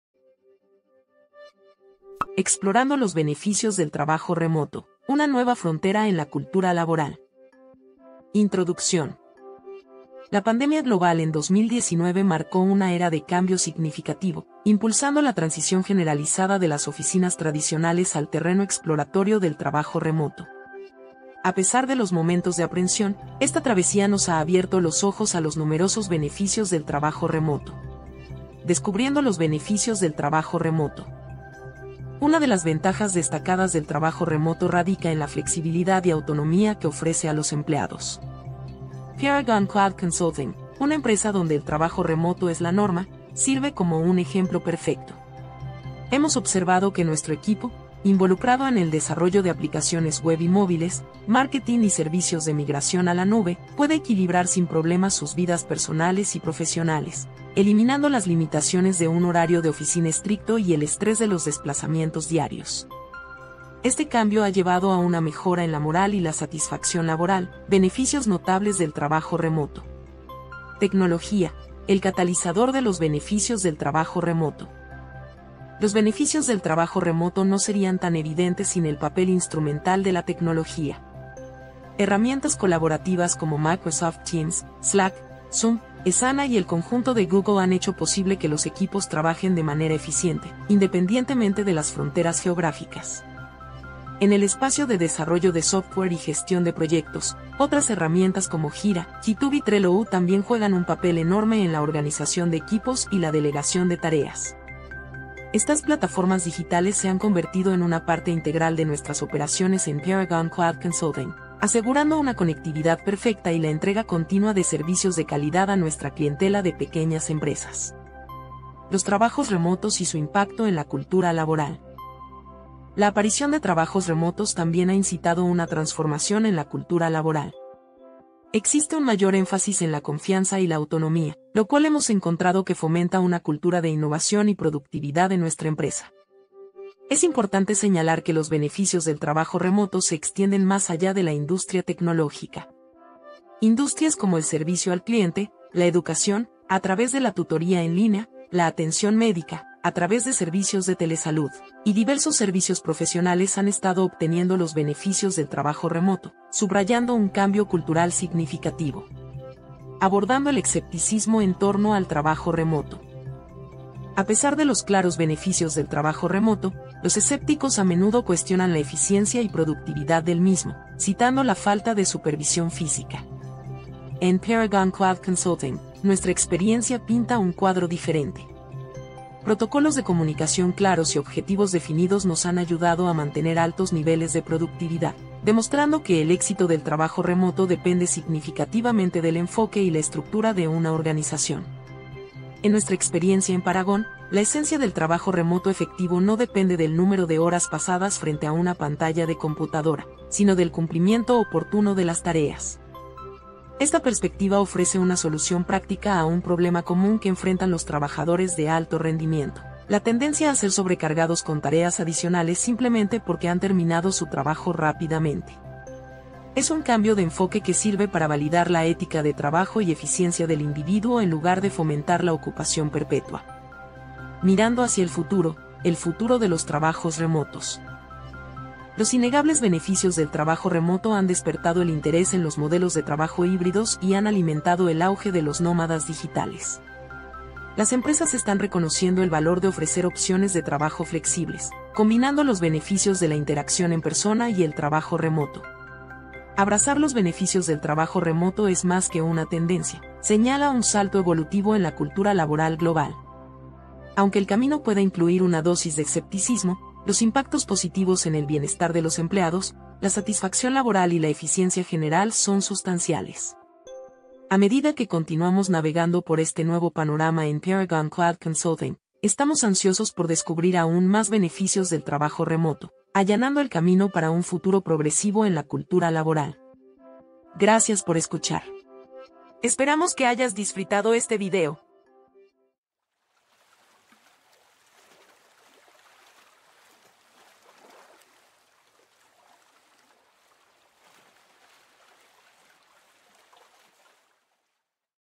Explorando los Beneficios del Trabajo Remoto: Una Nueva Frontera en la Cultura Laboral
💼🌍 ¡Bienvenido al mundo del trabajo remoto! 🚀 En este video esclarecedor, analizamos los numerosos beneficios del trabajo remoto, aprovechando nuestra propia experiencia en Paragon Cloud Consulting. 🖥️🌐

Desde la flexibilidad hasta la mejora de la satisfacción de los empleados, hemos presenciado en primera persona la transformación positiva que el trabajo remoto aporta a la cultura laboral. Como empresa de tecnología que ha adoptado el trabajo remoto, también compartimos cómo herramientas digitales como Microsoft Teams, Slack y Asana son clave para hacer del trabajo remoto un éxito. 🛠️💻

¿Todavía escéptico? Lo entendemos. 🧐 Por eso abordamos algunas de las preocupaciones comunes sobre la productividad y la falta de supervisión en el trabajo remoto, y compartimos cómo nuestro enfoque en la comunicación y la definición de objetivos nos ha ayudado a mantener una alta productividad. 🎯⏰

¡No te pierdas nuestra discusión sobre el futuro del trabajo remoto, incluyendo el auge de los modelos de trabajo híbrido y los nómadas digitales! 🗺️🌅

Ya seas un empleador buscando ofrecer opciones de trabajo más flexibles o un empleado preguntándote qué podría significar el trabajo remoto para tu equilibrio entre la vida laboral y personal, este video es imprescindible. 📺💡

👉 ¡Sumérgete para descubrir el poder transformador y el futuro del trabajo remoto! 🚀

Foto: Pexels.com @viktoria-slowikowska